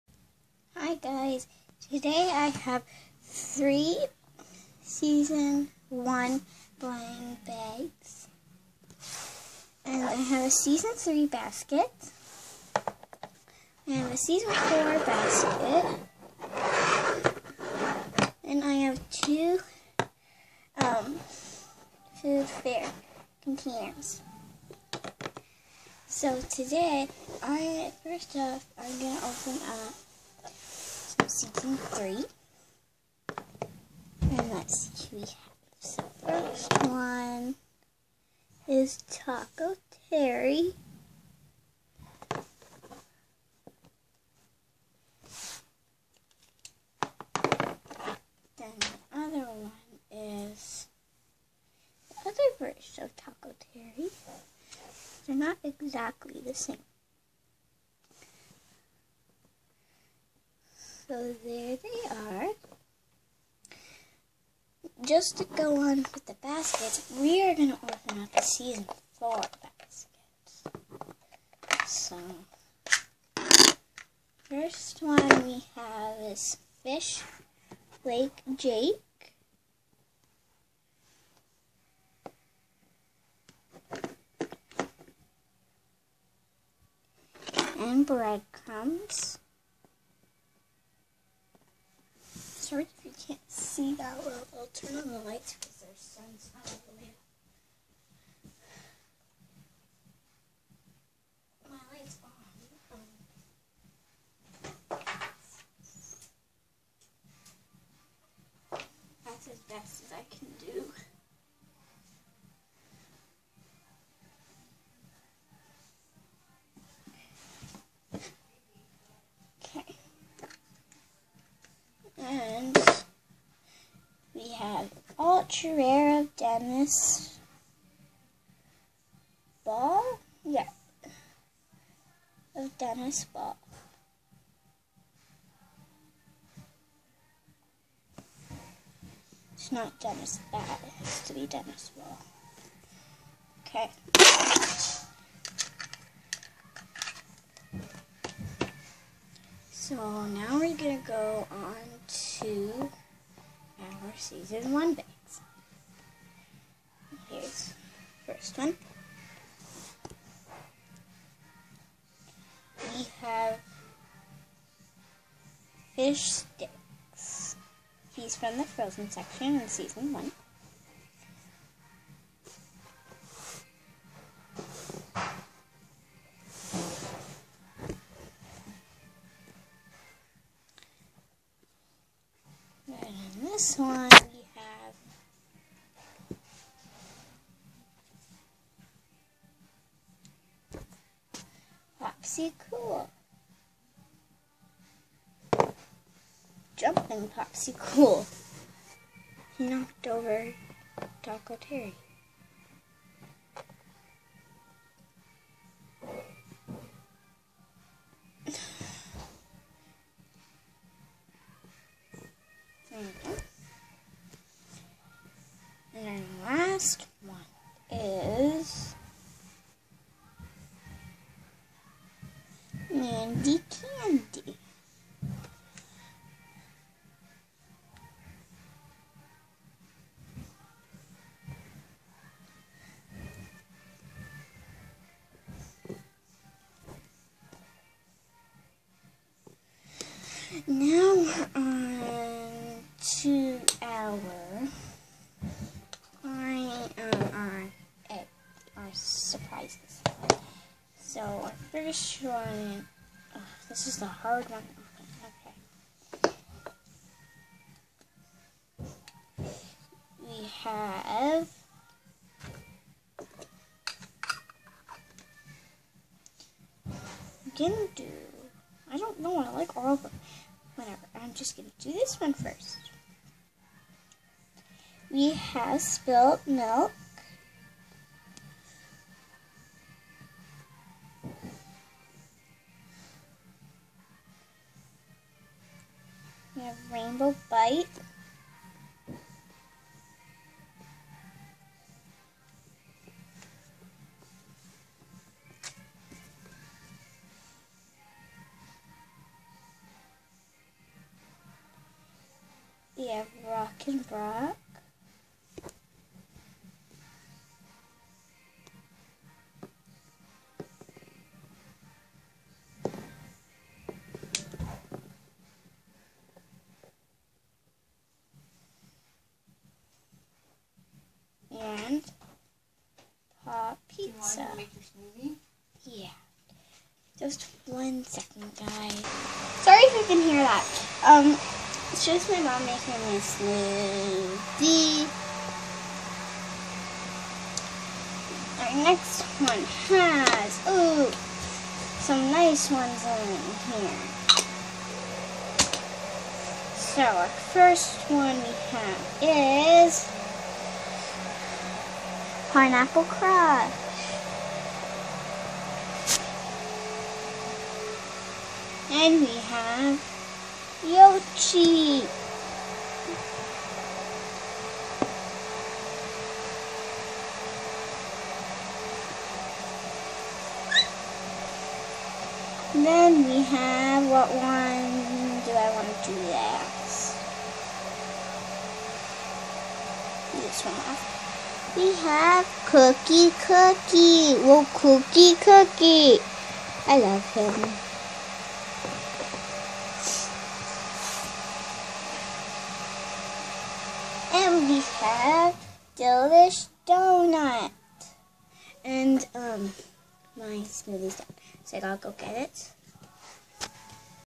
Abby is opening up 2 big suprise eggs and 3 seson 1 bags and 1 seson three basket and 1 seson 4 bask
Shopkin Toy Reviews